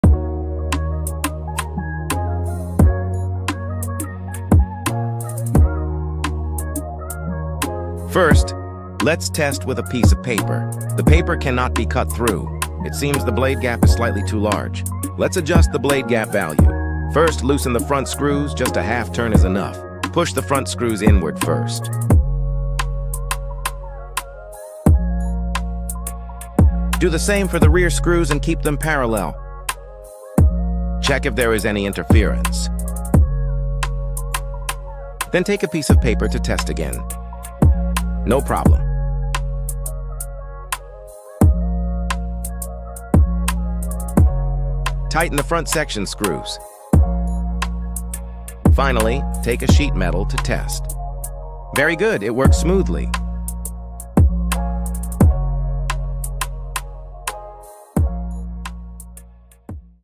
How to Adjust the MMS-4 Throatless Shear blade gap
Loosen the lower blade's clamp bolts, then use adjustment screws to set the gap based on the material thickness—typically about \(1/4\) the material thickness, or a feeler gauge reading—before re-tightening the clamp bolts. Fine-tune by inserting scrap material to test the cut and making small adjustments with the screws until there are no burrs or binding.